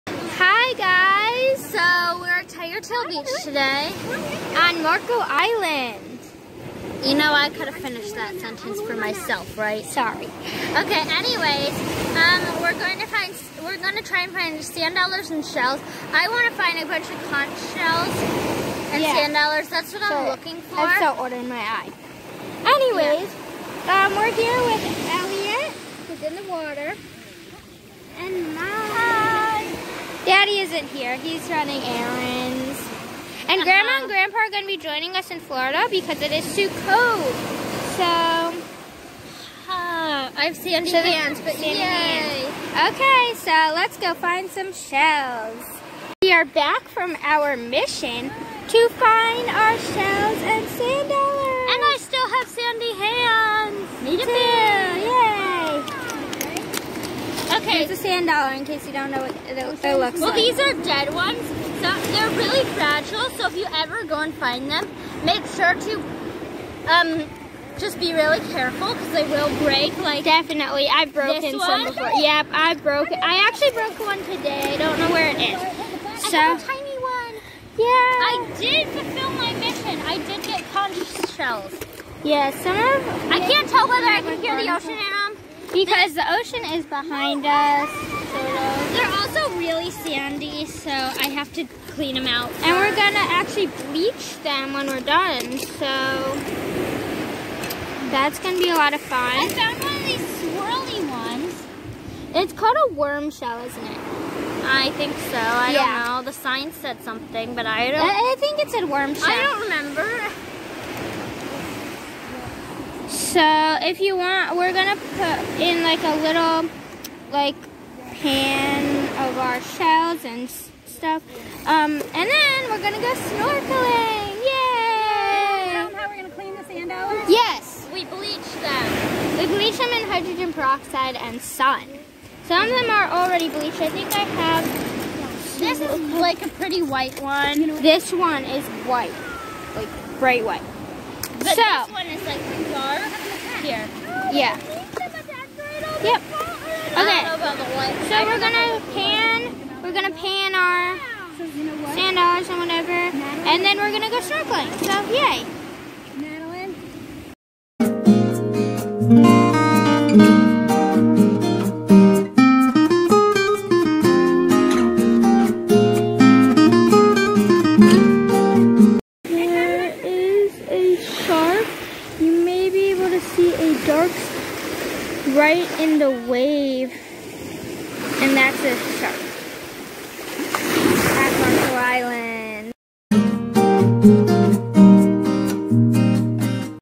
Finding Sand Dollars on Marco Island | The Zwick Sisters | #10
In today's video, we go hunting for sand dollars and sea shells. We also saw a shark in the water! We hope you enjoyed.